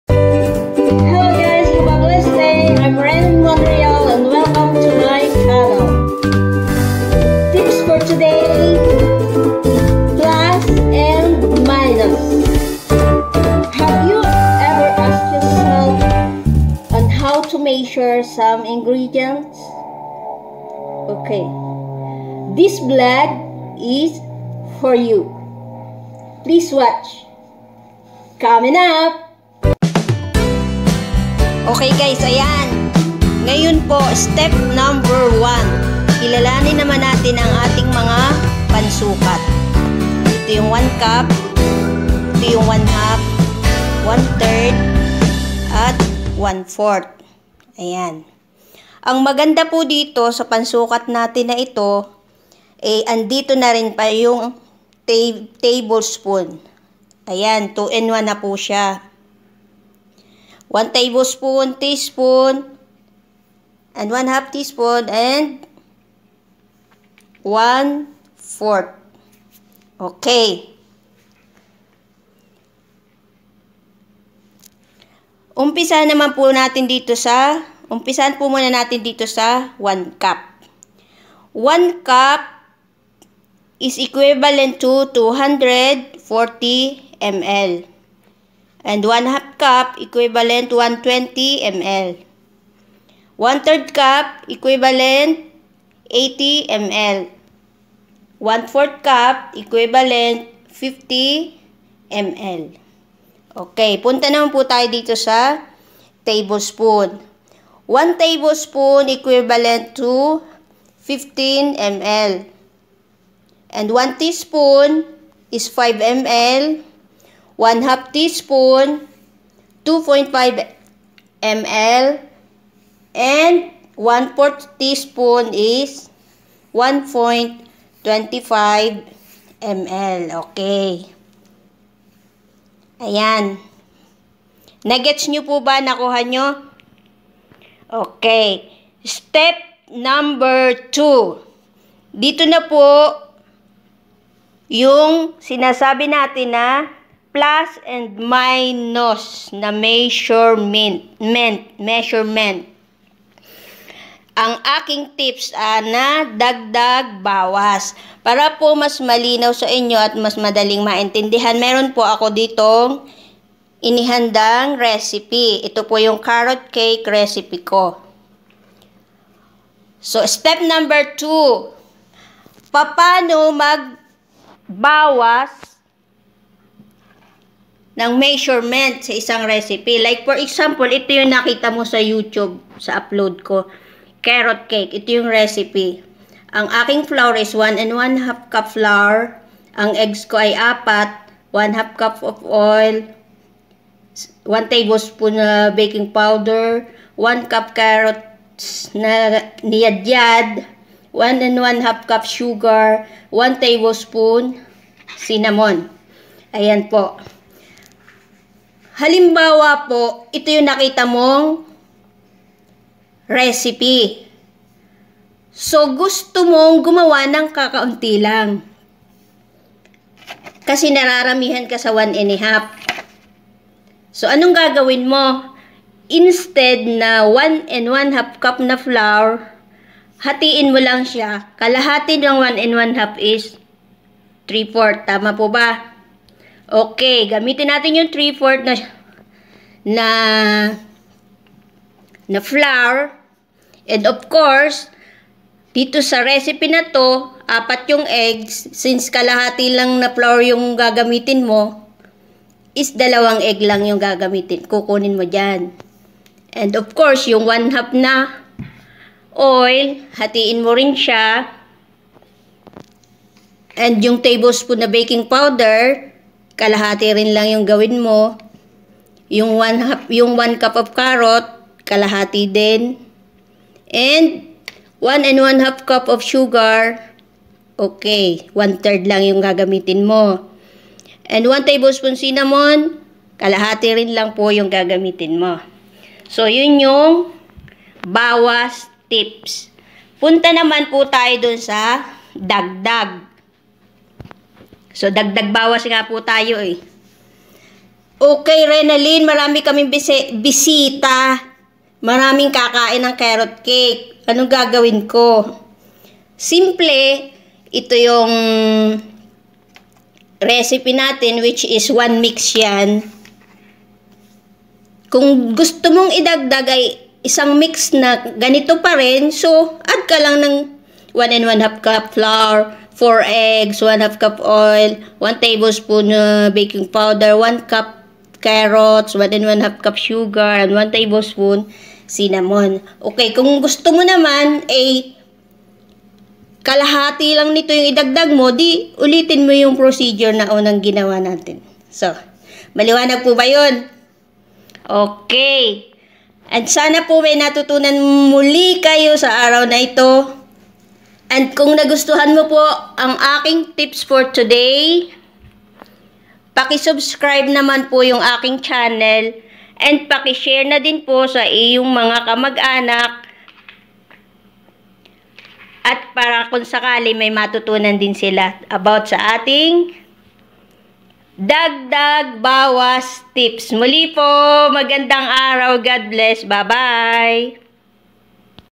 Renalyn Monreal's | Kitchen Tips Ideas
To help people understand on how to measure the ingredients in recipe. Get familiar to measuring tools and it's unit

Find me in Facebok
Renalyn Rodelas Monreal

Renalyn Rodelas